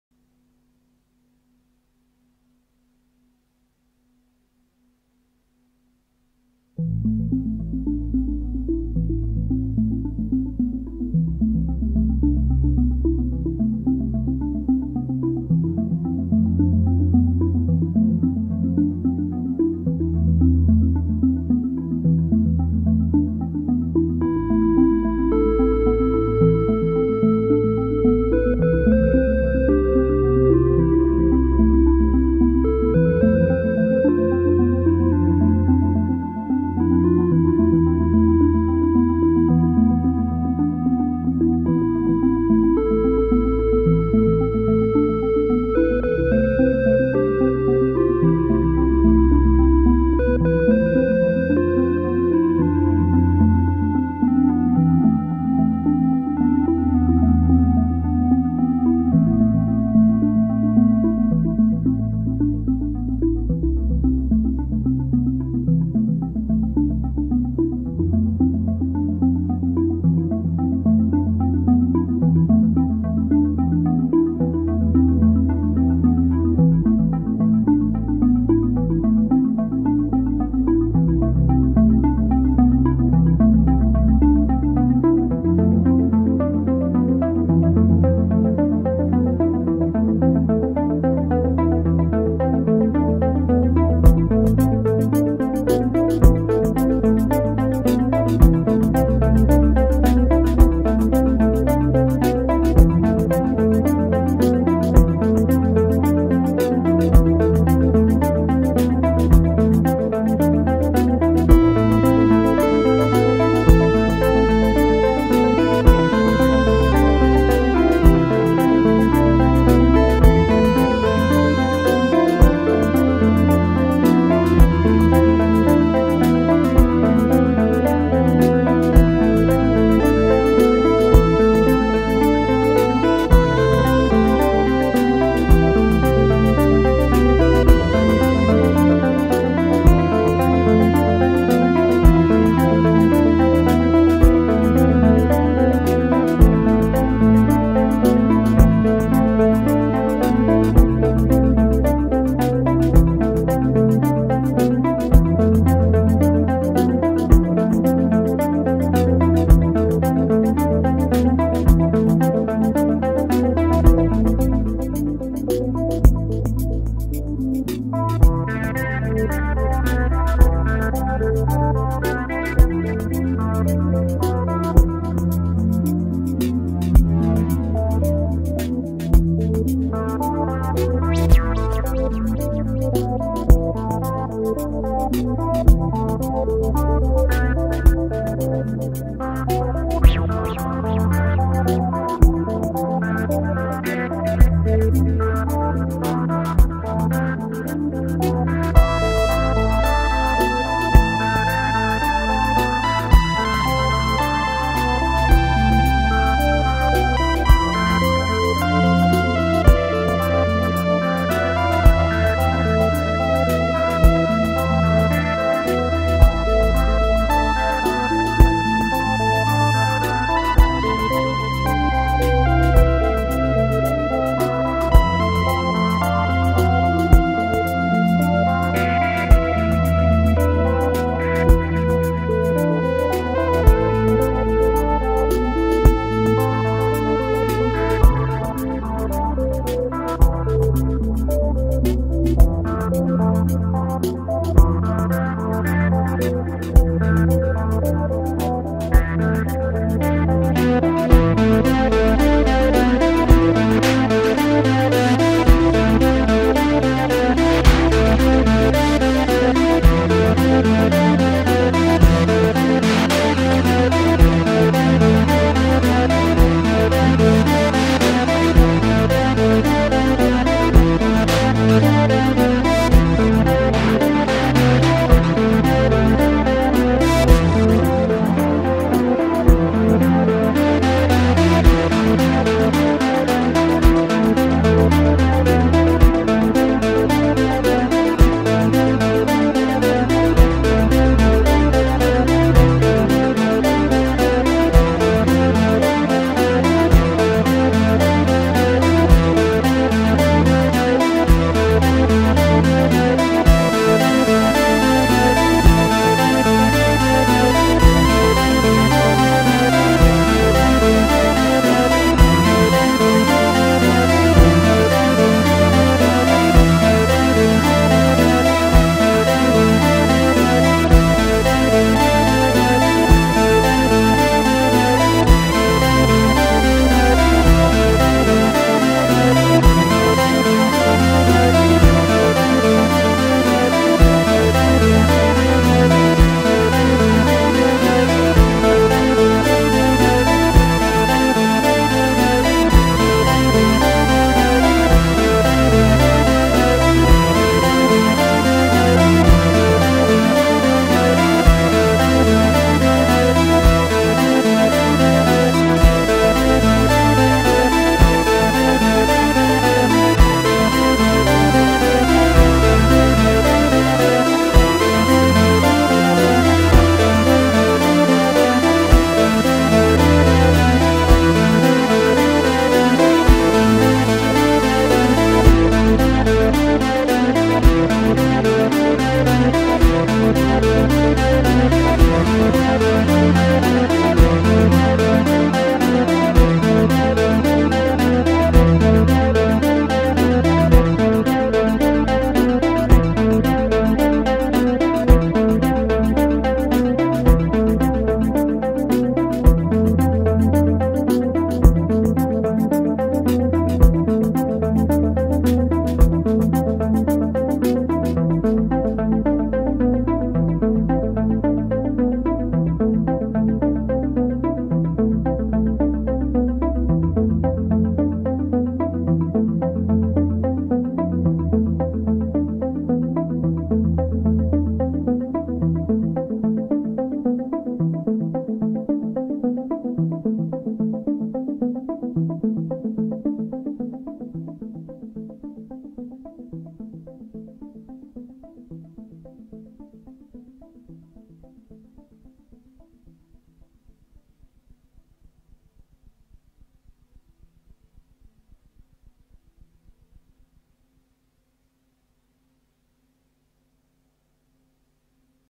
Zerøbeat - Improvisation with the Odyssey
A quick improvisation using the Behringer Odyssey via the Specular Tempus. Also featuring the Korg Minilogue (bass) and the Roland SH-01A (lead). Drums are the Korg Volca Beats via the Mini Kaoss Pad 2.